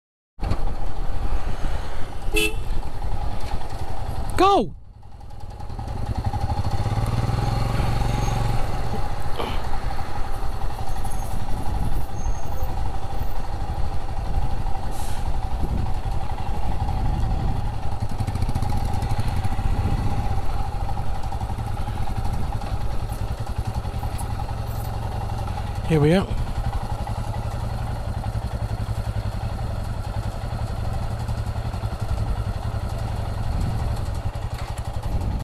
Arrival at Riverside Hotel Srinagar (1)
this is a video compilation of our trip along Highwy101.and highway 7 to Sringagar on our Himalayans, it was 88km and took 3 hrs